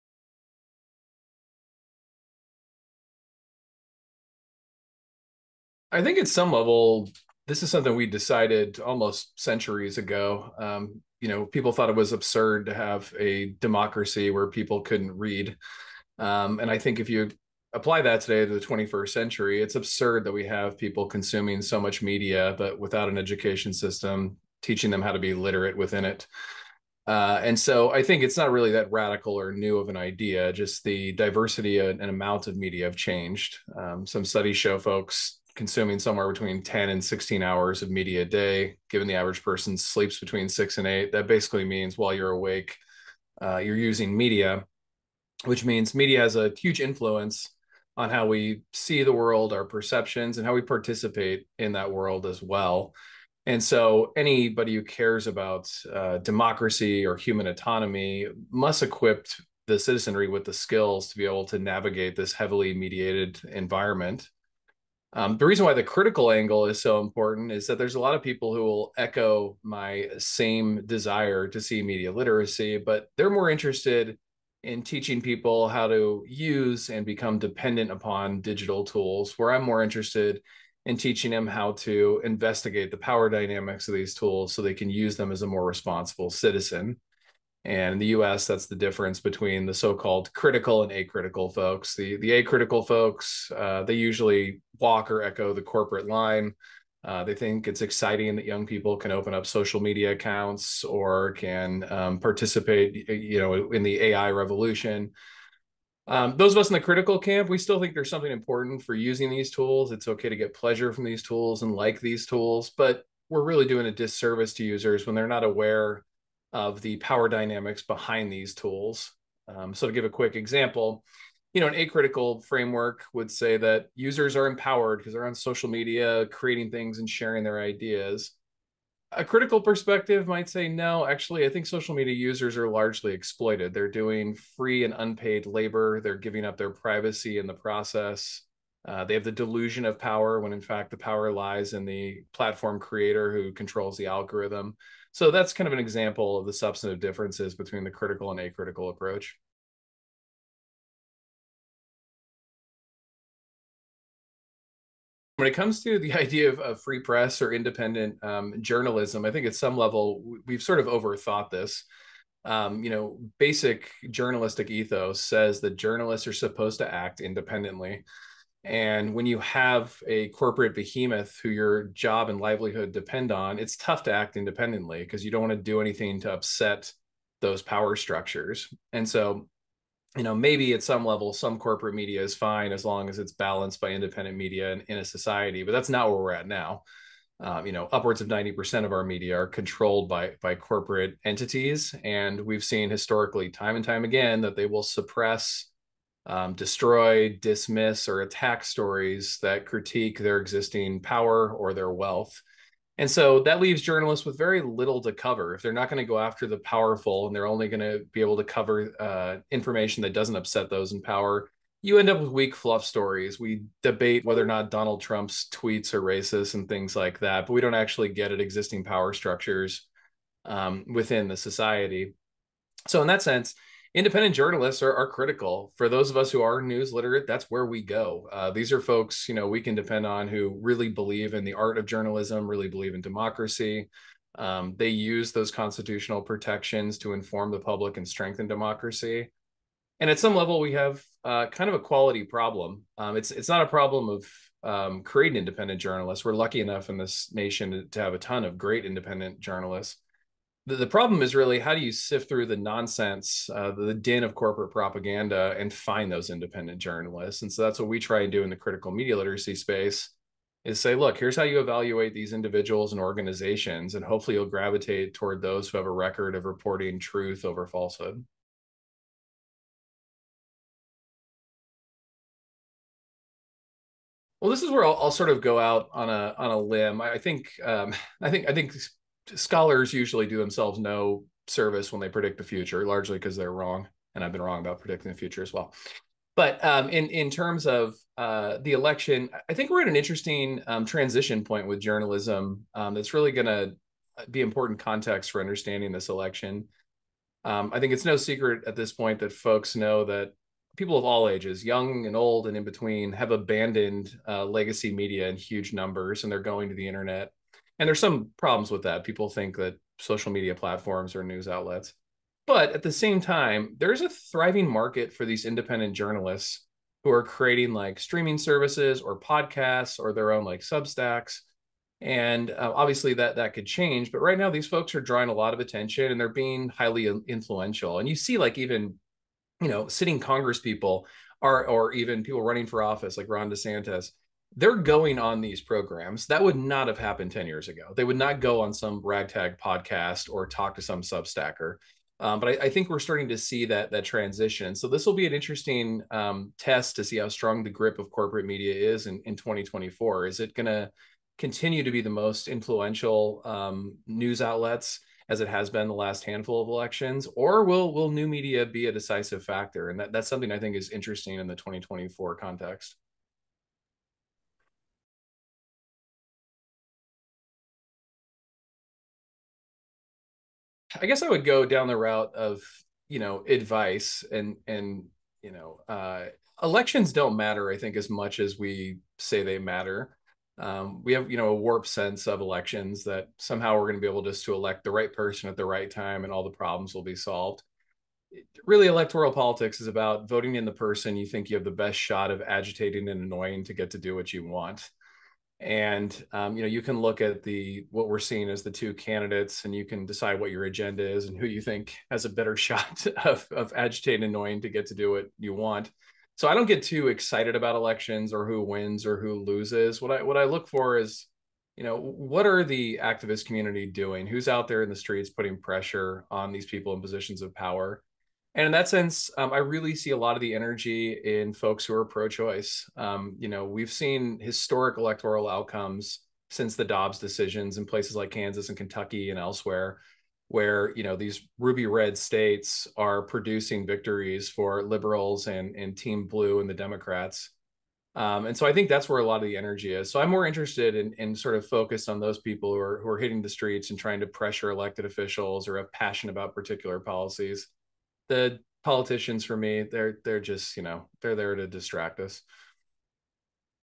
Decoding Democracy: Nolan Higdon
A collection of noted media scholars, journalists, and activists joined Project Censored to discuss the importance of critical media literacy education, independent journalism, and civic engagement during an election year. This collection of interviews highlights how an informed public can safeguard democracy from corporate interests, social media algorithmic manipulation, establishment press distractions, and various forms of censorship and work to build a more just and inclusive society. Through these conversations we aim to empower individuals with the knowledge and tools required to meaningfully navigate today’s complex media landscape and hyper-partisan political climate to become more well-informed and engaged citizens.

Special thanks to the Steering Committee of the Union for Democratic Communications (UDC); and the hosts of their conference “Left Undone,” the Media, Inequality, and Change (MIC) Center, a collaboration between the University of Pennsylvania’s Annenberg School for Communication and Rutgers University’s School of Communication and Information; and the Project Censored team for making this series possible. Most of these recordings took place during the UDC conference at Penn between October 12-15, 2023 (unless otherwise indicated).

You can view all of the Decoding Democracy videos on our

Many of the interviewees gave detailed responses, but for the purposes of this video compilation, these were edited for concision, cohesiveness, and to better conform for an educational environment. The complete interviews will be available to view on the official Project Censored YouTube page.